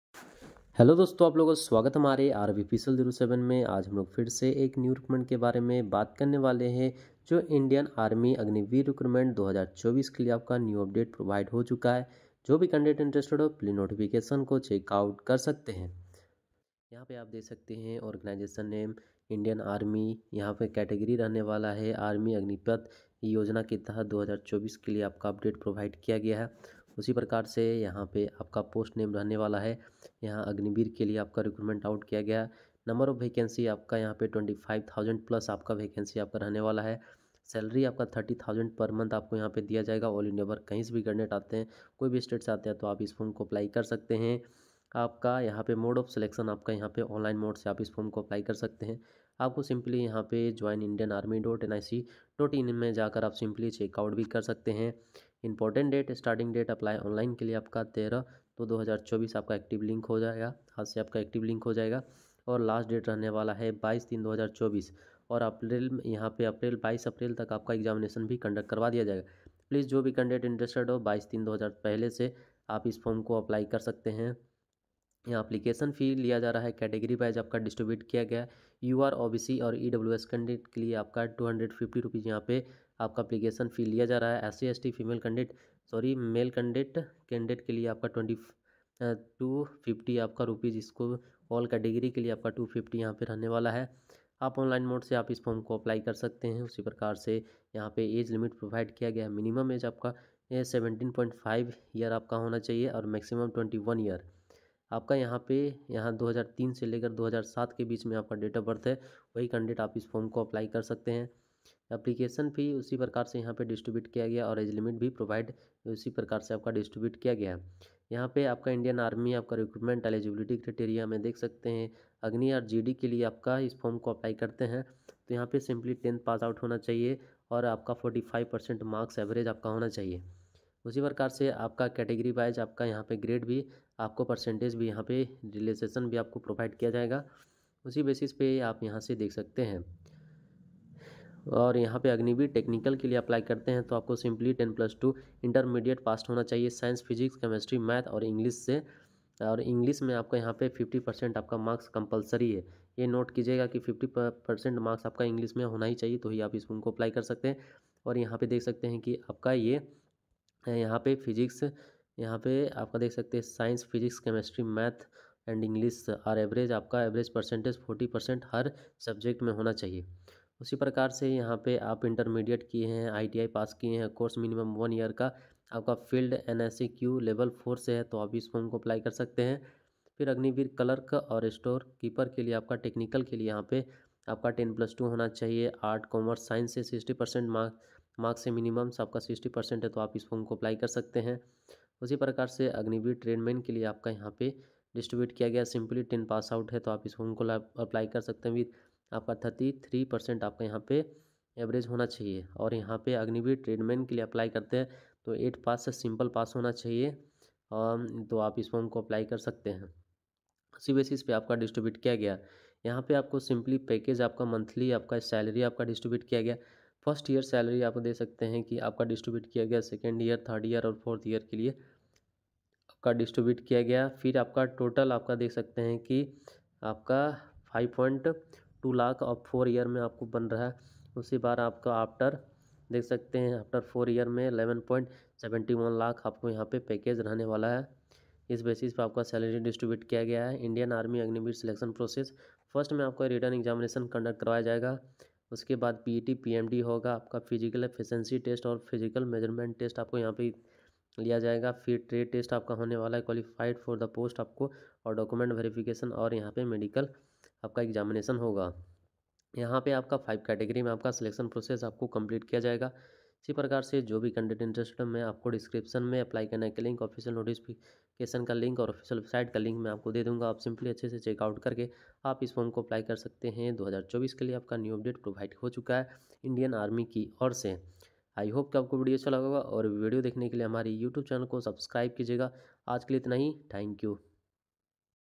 Indian Army 🪖 Agniveer Rally Recruitment 2024 || Online From Out || How To Fill Rally Recruitment
Indian Army Agniveer Recruitment 2024 Rally Schedule Notification

Indian Army Agniveer Recruitment 2024: Indian Army has Invited Online Application Form for the Recruitment of Agniveer under Agnipath Scheme 2024. Those Candidates are interested in the Following Process of Indian Army Agniveer Recruitment 2024 & complete the Required Eligibility Criteria Can read the Full Notification and Apply Online for Army Agniveer Recruitment 2024 and Indian Army Agniveer Rally Schedule 2024 joinindianarmy.nic.in